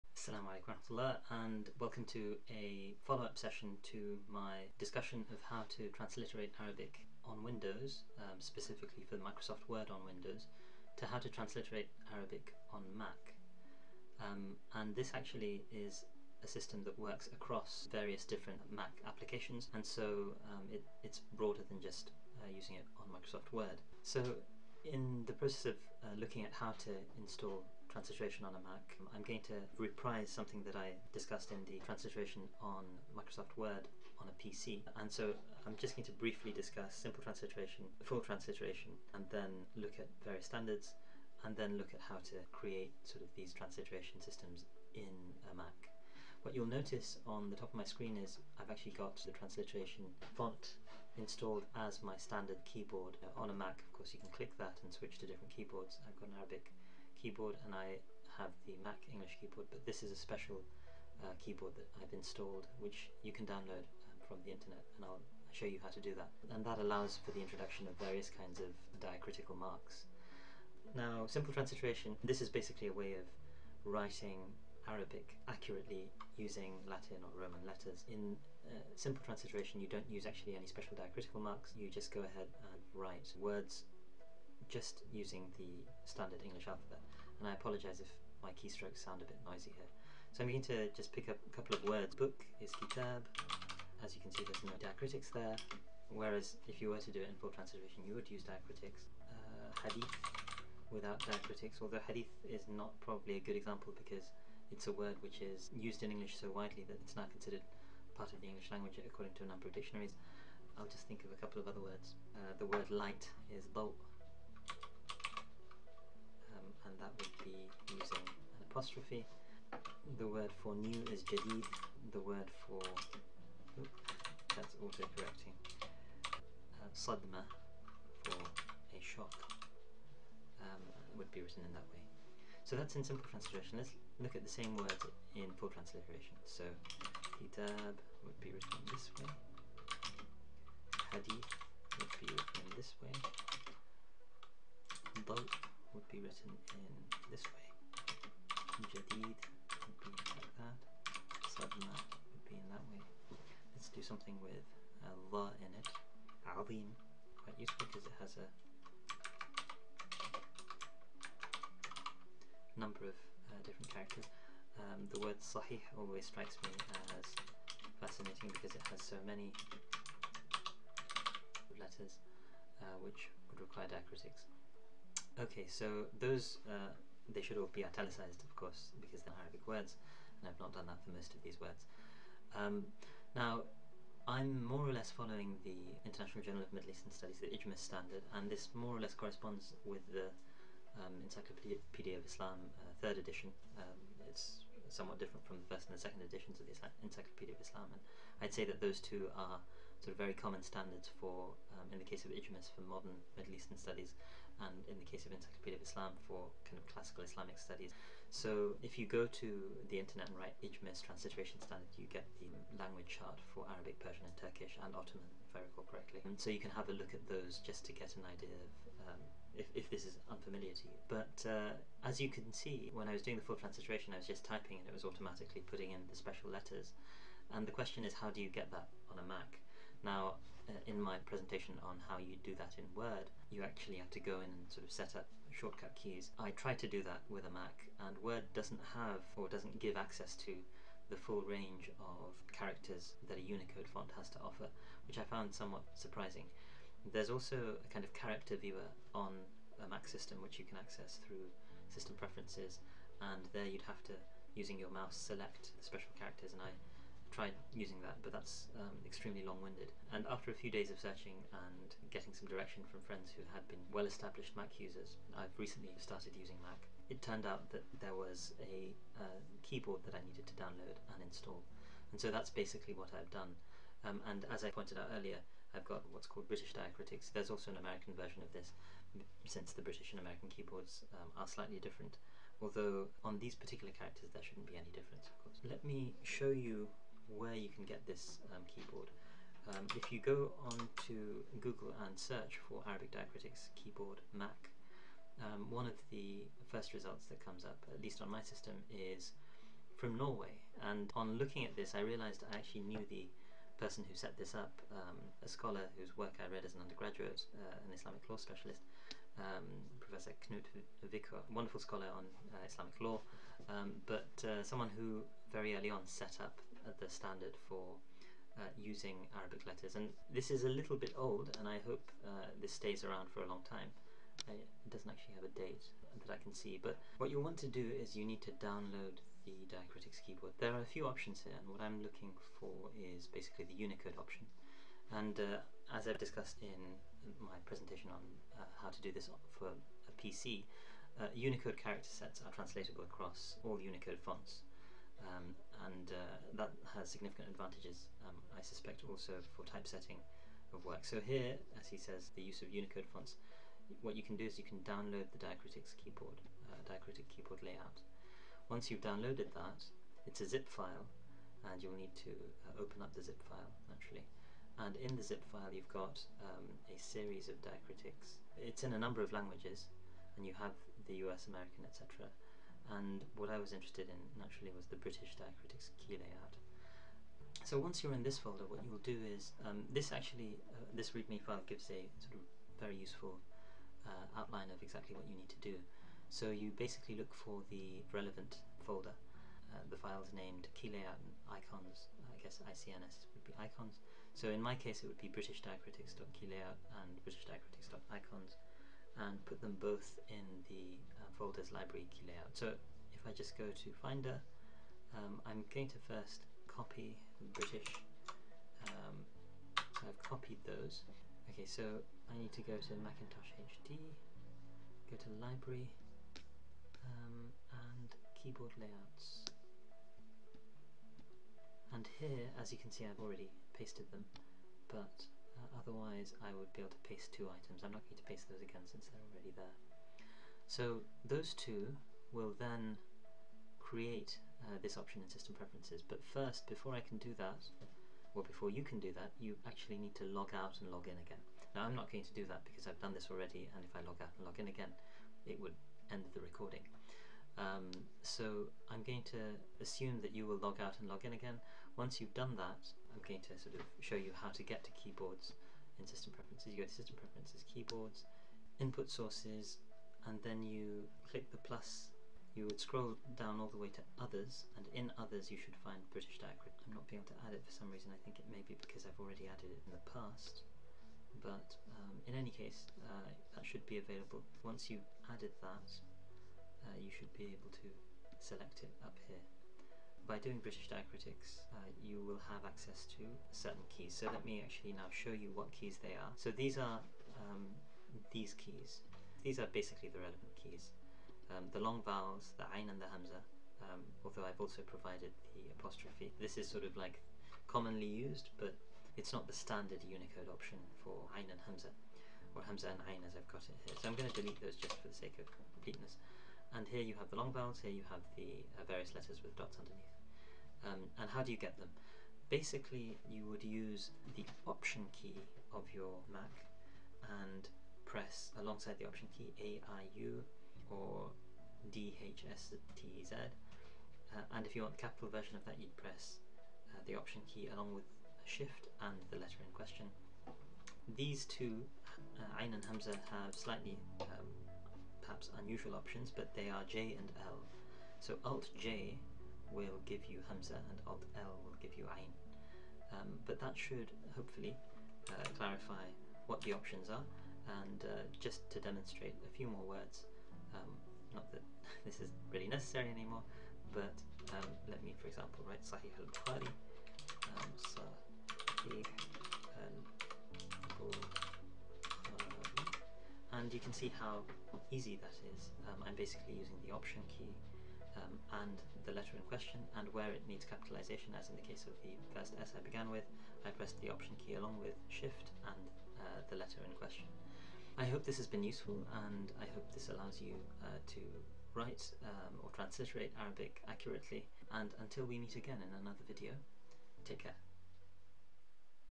How to Use Diacritics on a Mac when Transliterating Arabic
A guide to installing shortcuts and using diacritics when writing Arabic transliteration on a Mac